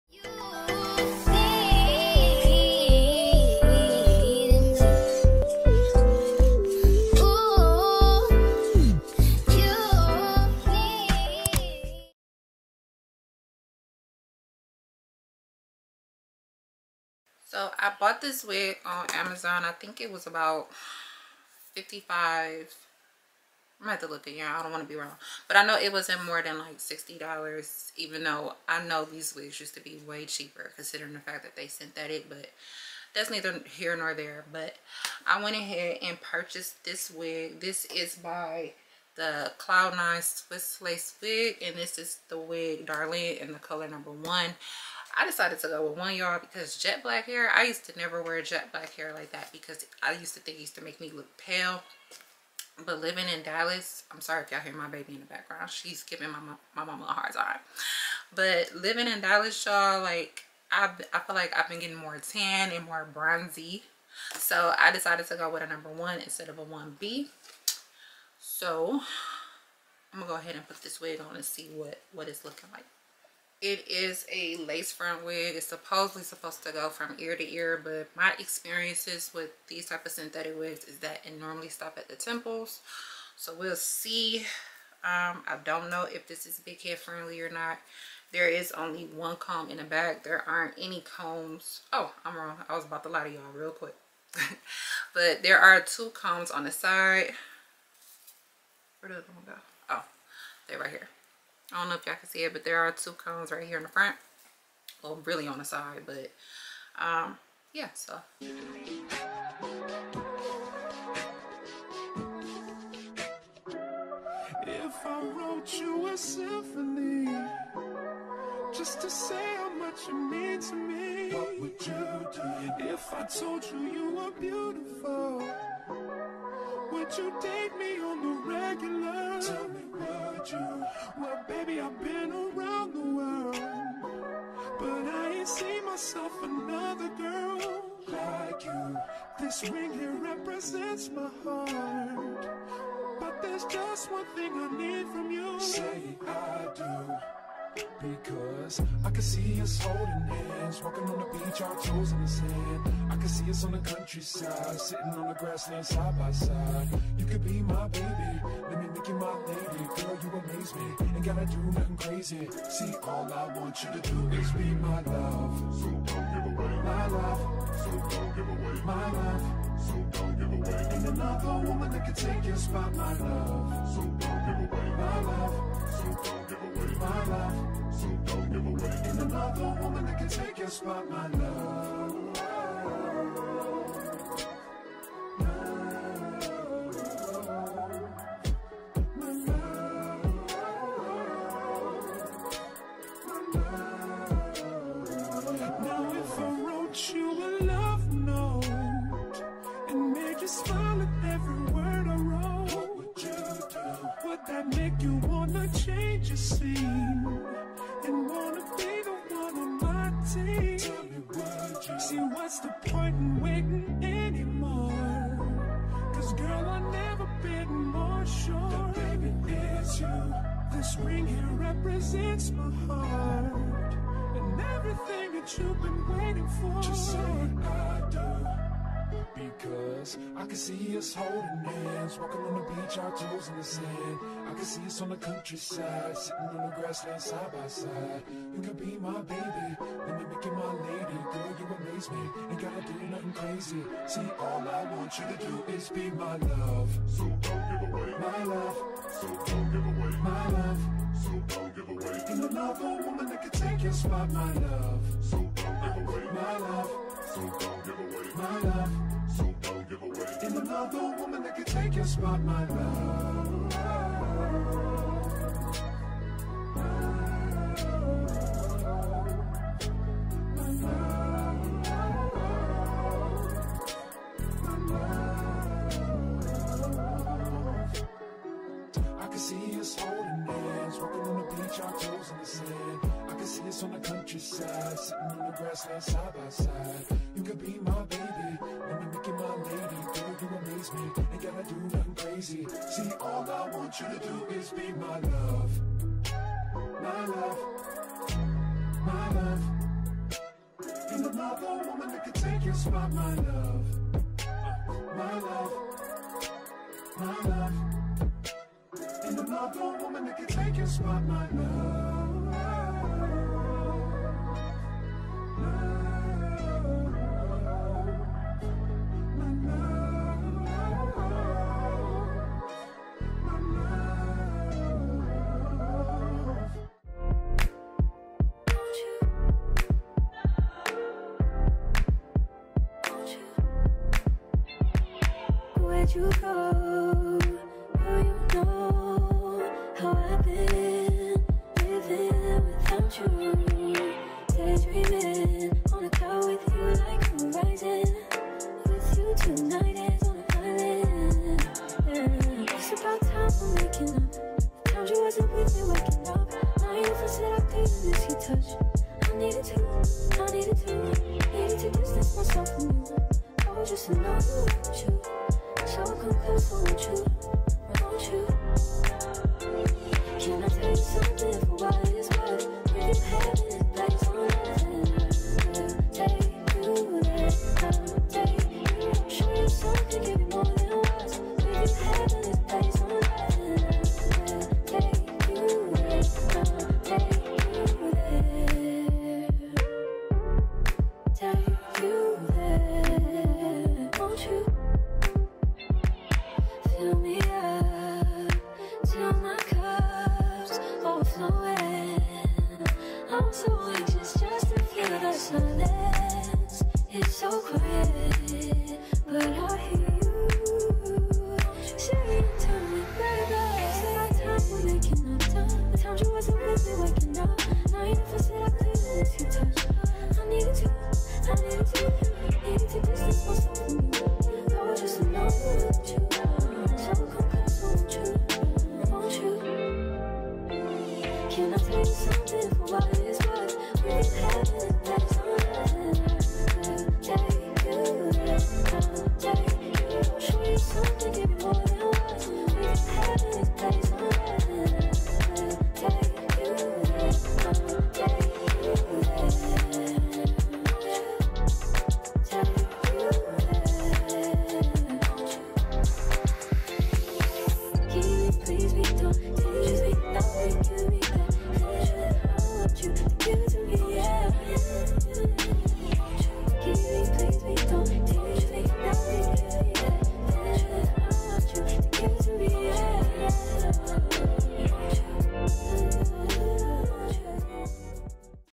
Sensationnel What Lace DARLENE Wig ft. Amazon Prime | It's The Curls For Me
Hey frieeennnddss and welcome back! I decided to do a wig review on this pretty wig by Sensationnel What Lace - Darlene. Definitely a wig to wear for a night out but not an everyday wig!

PRODUCT: Sensationnel What Lace - Darlene Wig Color 1 (Purchased on Amazon)